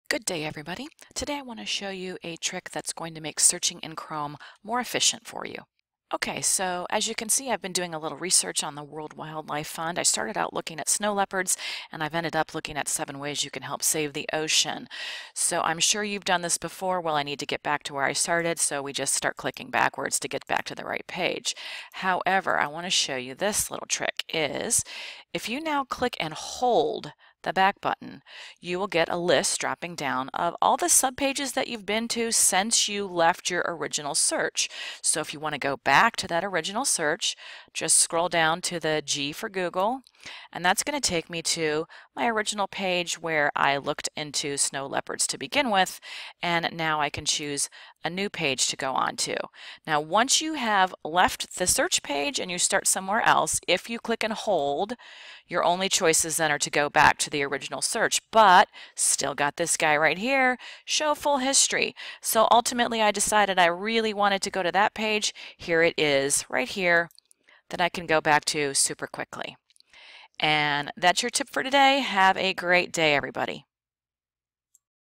Stop Clicking that Back Button!
There are more efficient ways to find your way back to a page you've previously been.